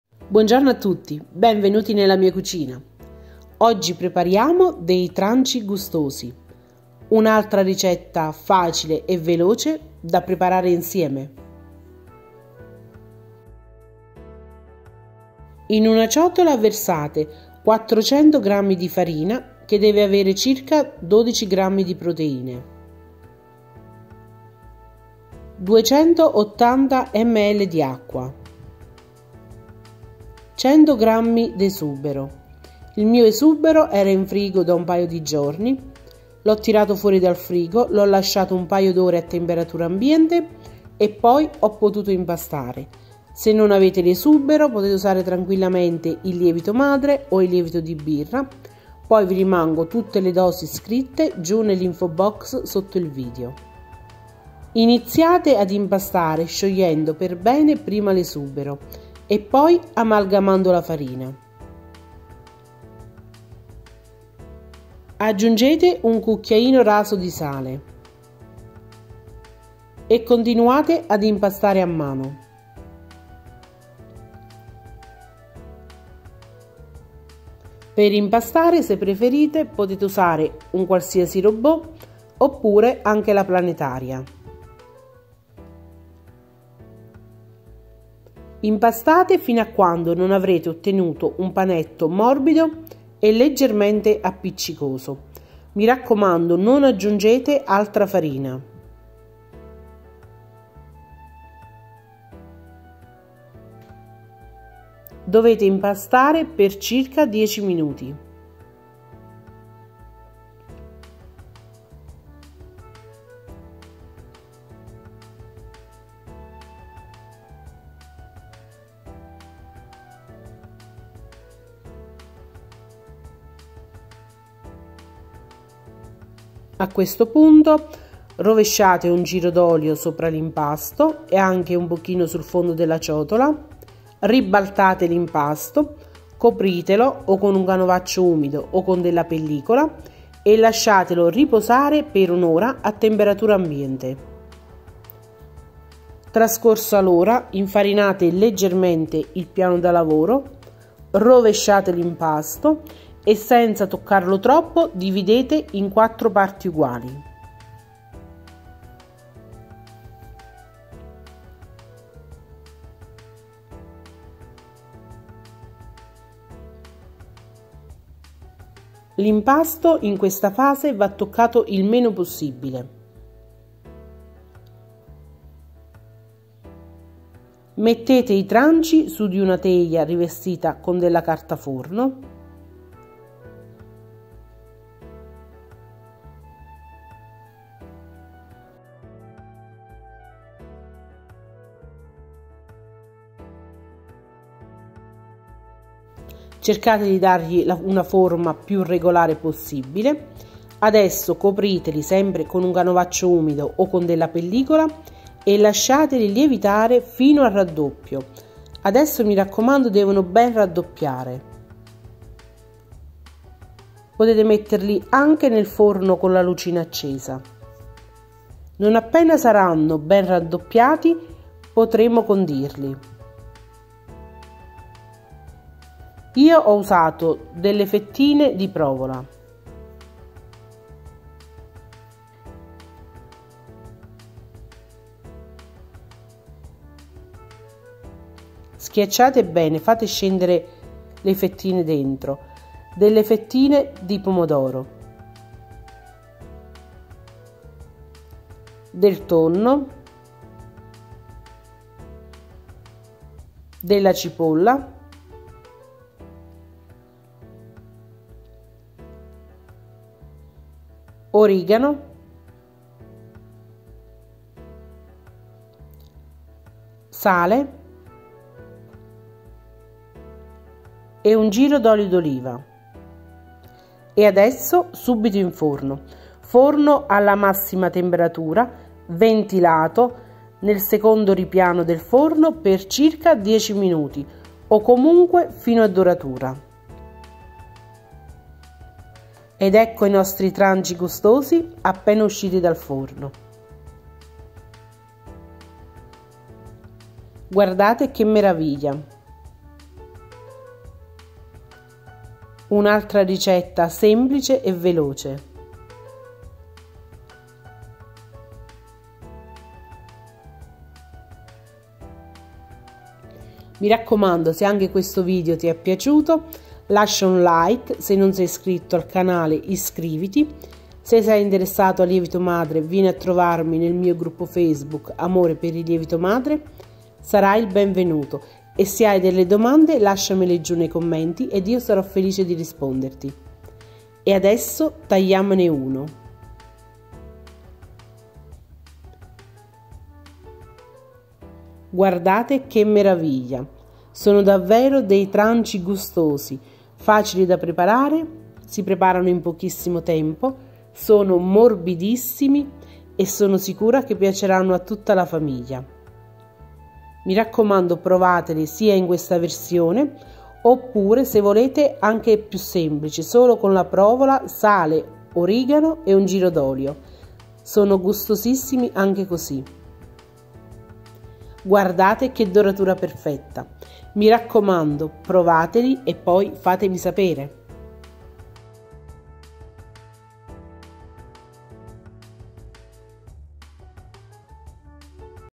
Tranci gustosi
Buongiorno a tutti. Oggi prepariamo dei tranci gustosi, una ricetta facile e golosa!

Ingredienti per 4 tranci:

400 gr di farina con 12 gr di proteine,
280 ml d' acqua,
100 gr d' esubero oppure
100 gr di lievito madre

 oppure
fate un lievitino con: 60 gr di farina, 30 ml d' acqua e 5/6 gr di lievito di birra,

1 cucchiaino raso di sale.

Per condire:

provola,
pomodorini,
tonno,
cipolla,
origano,
sale,

Cottura:

forno ventilato,
massima potenza,
per circa 10'